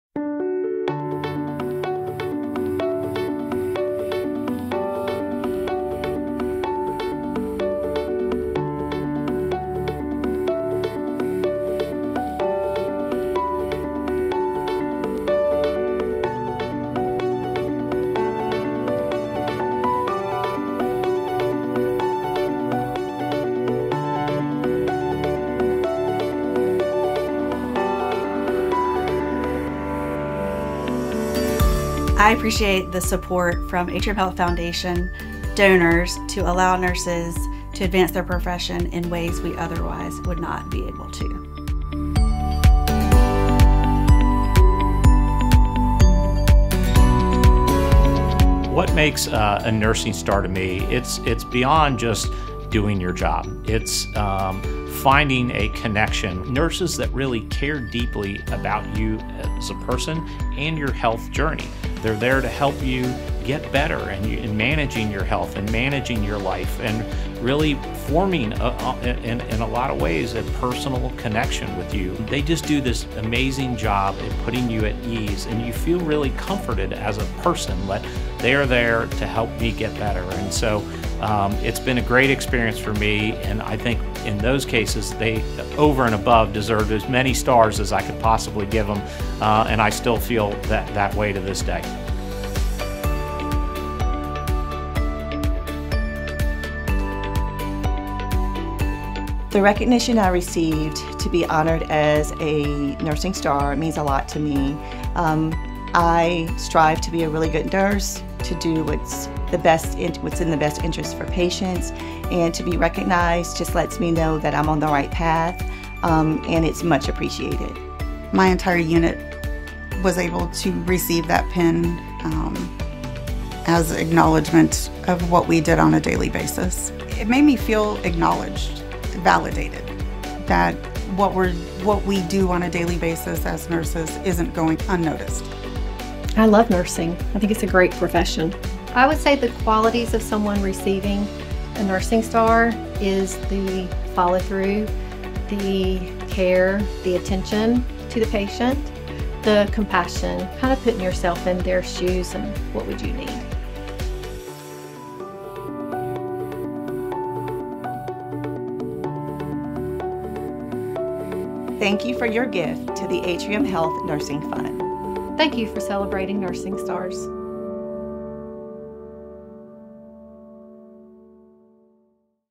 A Look at the Atrium Health Nursing Stars program
Established in 2019, the Atrium Health Nursing Stars program provides former patients and generous donors the opportunity to recognize outstanding nurses and advance the nursing profession through philanthropy. Donations support continuing education, research, and professional development opportunities for Atrium Health nurses in the Greater Charlotte area.